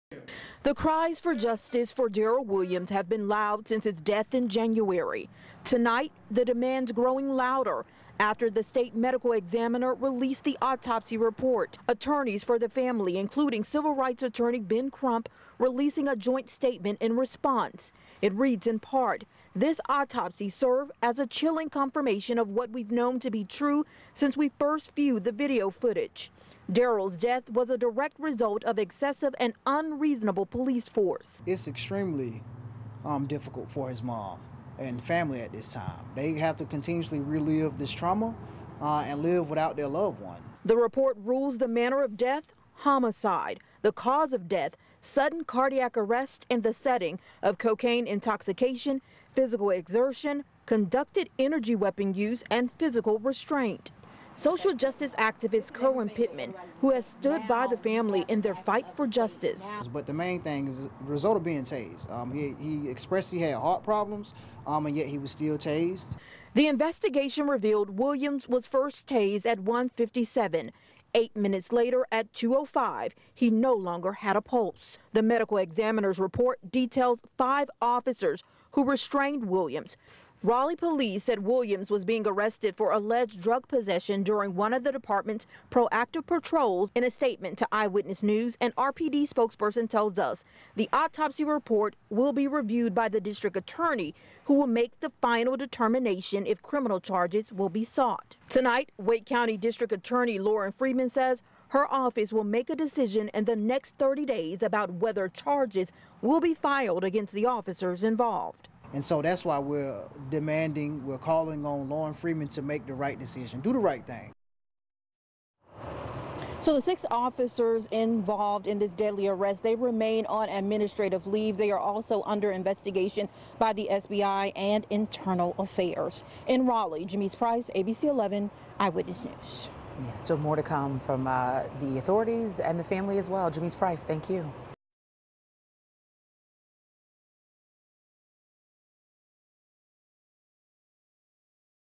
Autopsy lists cardiac arrest as cause of death for Darryl Williams, man who died in police custody
The full autopsy report for Darryl Williams, who died in police custody, has been released. It lists manner of death as homicide.